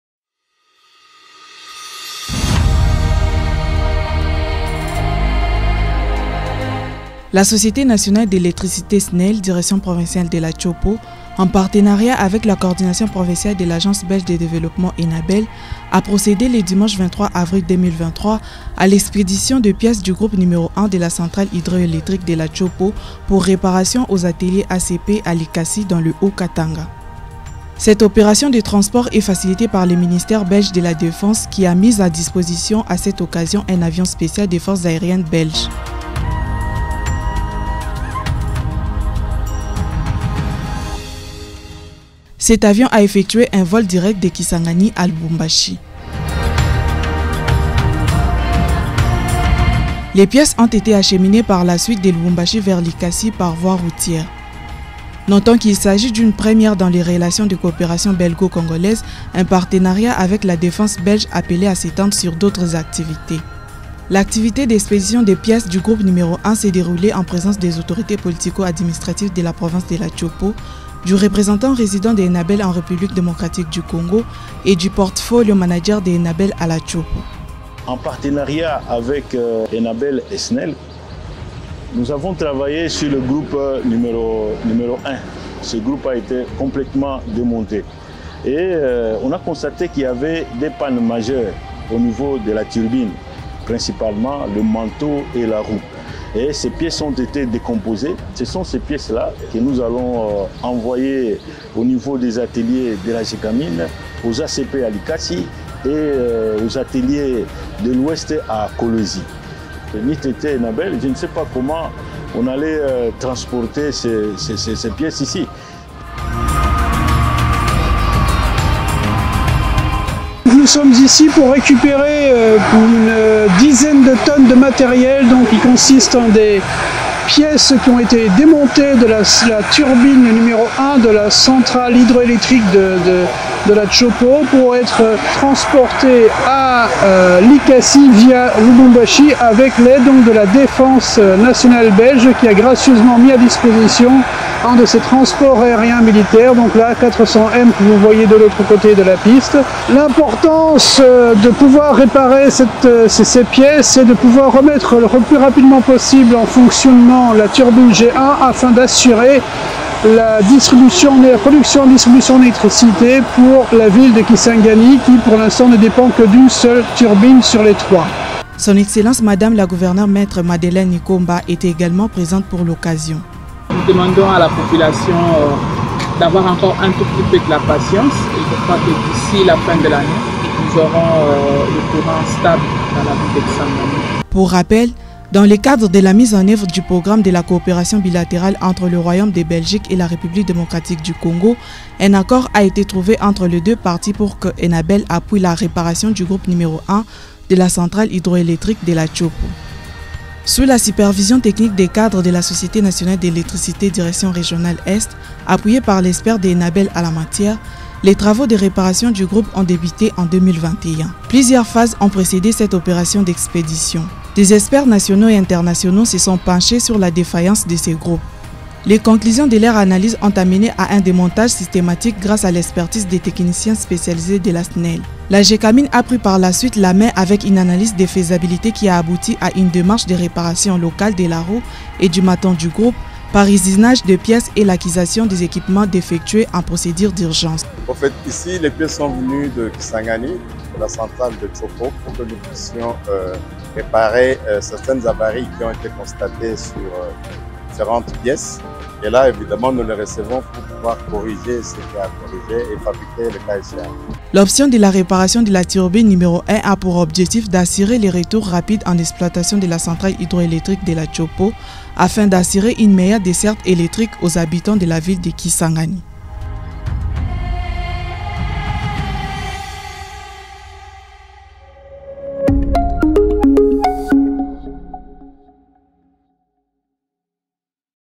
Démarrage des travaux de réparation du Groupe G1 de la centrale hydroélectrique de la Tshopo
Une synergie sans précédent, des moyens hors normes, pour que les habitants de la Tshopo recouvrent l’électricité. Enabel en RDC a travaillé avec la Société Nationale d’Electricité – SNEL a procédé à l’expédition des pièces du groupe n°1 de la centrale hydroélectrique de la Tshopo le 23 avril 2023, pour réparation aux ateliers ACP de Likasi dans le Haut-Katanga.

Le Ministère Belge de la Défense a, pour l’occasion, mis à disposition un avion des forces aériennes belges.